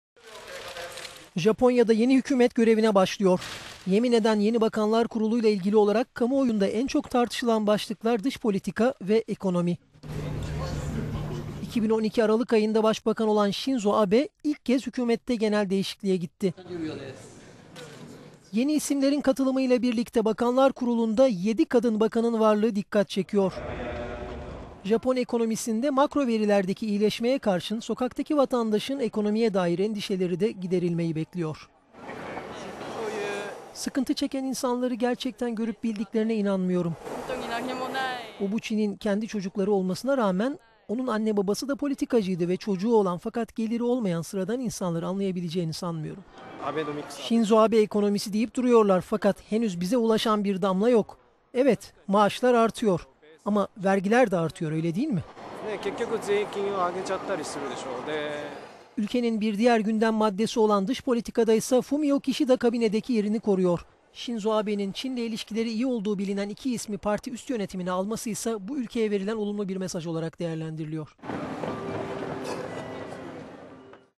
Japonya'da II. Abe hükümetinin ilk gündemi Çin ve ekonomi
Japonya'da yeni hükümet görevine başlıyor. Yemin eden yeni bakanlar kurulunda en çok tartışılan başlıklar dış politika ve ekonomi. Abe 2012 Aralık ayında başbakan olmasından bugüne ilk kez hükümette genel değişlime gitti. Yeni isimlerin katılımıyla kurulda yedi kadın bakan bulunduğu dikkat çekiyor. Makro ekonomik verilerdeki, iyileşmeye karşın sokaktaki vatandaşlın ekonomik endişeleri giderilmeyi bekliyor:

"Sıkıntı çeken insanları gerçekten görüp bildikleine inanmıyorum. Obuchi'nin çocukları ol…